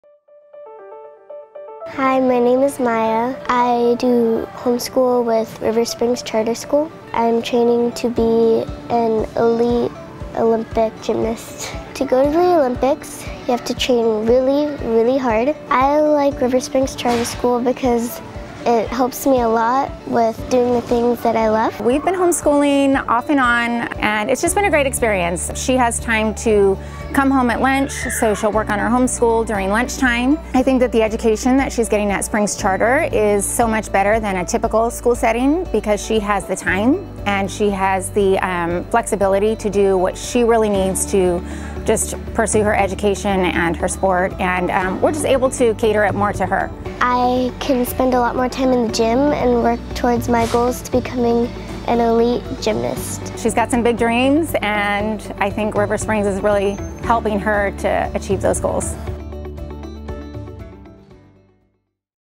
Student Spotlight - Maya Molina
Meet Maya! A Springs Homeschool student and an outstanding young gymnast. She has been involved with gymnastics for five years and has advanced to an impressive Junior Olympic level 8.

Maya has competed in the Talent Opportunity Program, has won at Level 7 regionals, and participated in a Developmental Invite Camp in Tennessee, training with the same coaches who coach the USA Gymnastics Women’s National Team.

Participation in Springs’ Homeschool program has given Maya the flexibility to pursue her passion for gymnastics. She hopes to go on to be a gymnast at UCLA and compete in the Olympics.